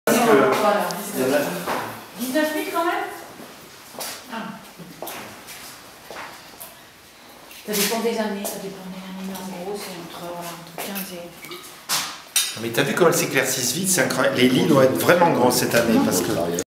Tasting the 2011 WHITE WINE from the casks, Château Lestage (Listrac, Bordeaux)
Mr. Jean Chanfreau, owner of Château Lestage and Château Fonréaud (Listrac, Bordeaux) tasting the 2011 white wine from the casks.

This wine is in the casks for ageing since one week.